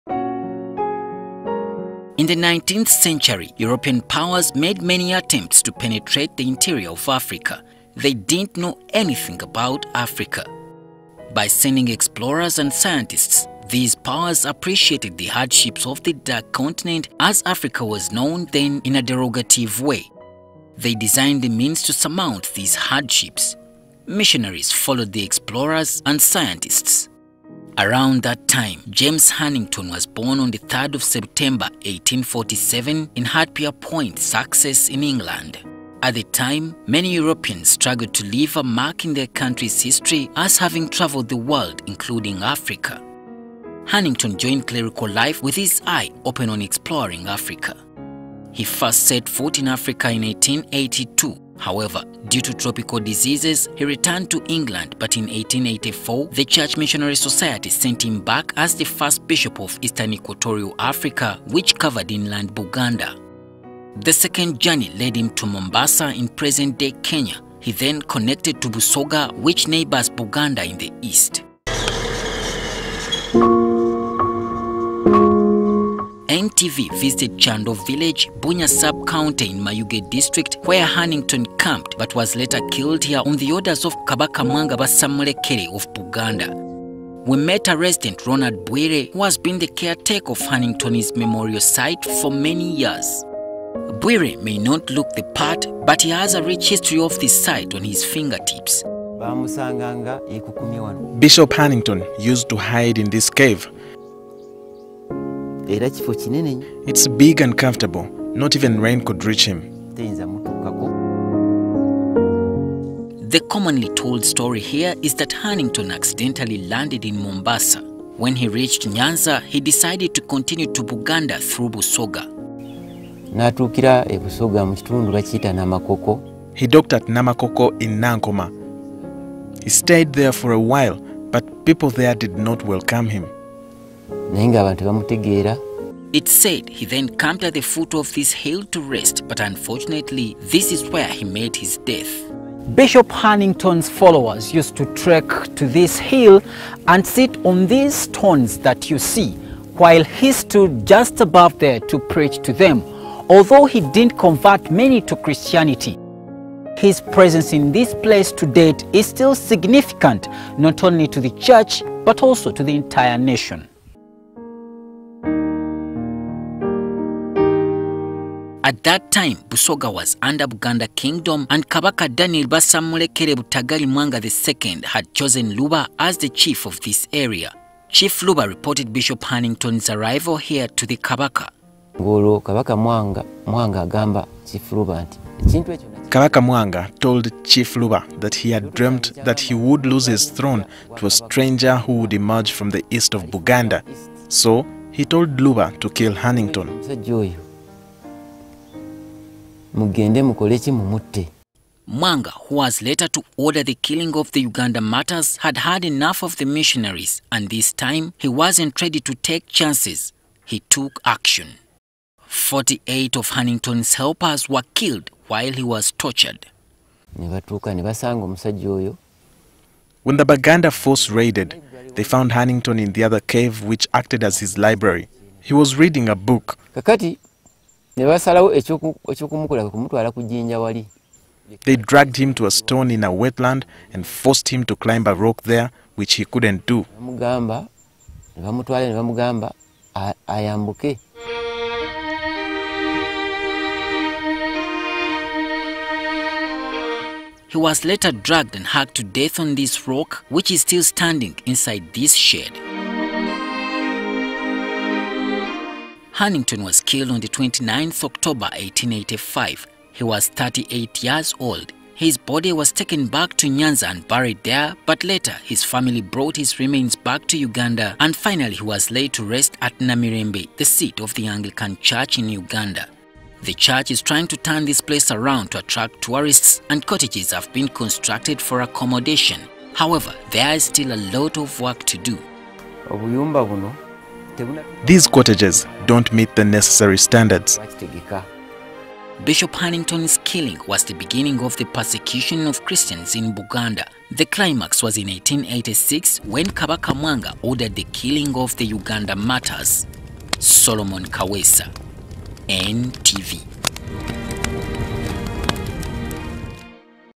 How Bishop James Hannington became Uganda's first Martyr
On 3rd June this Sunday, it will be the celebration of the Uganda Martyrs who were killed for their faith in 1886 in Buganda.

However, some accounts say first Martyr was a white man - Bishop James Hannington who was killed in Busoga on the orders of Kabaka Mwanga II of Buganda who also ordered the killing of the Uganda Martyrs.